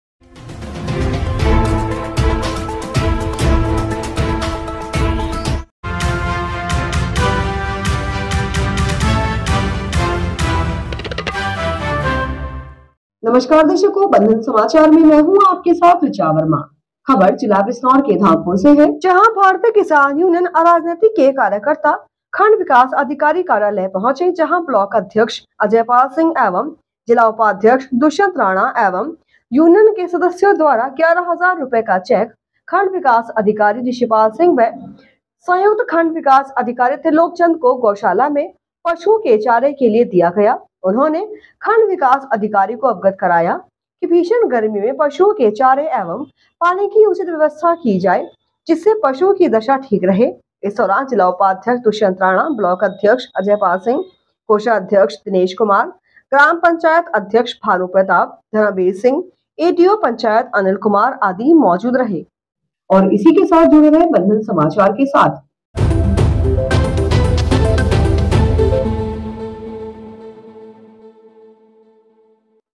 भाकियू अराजनैतिक ने गौशाला के लिए बीडीओ धामपुर को सौंपा 11 हजार रूप्ये का चैक
भारतीय किसान यूनियन अराजनैतिक के कार्यकर्ता खंड विकास अधिकारी कार्यालय पहुंचे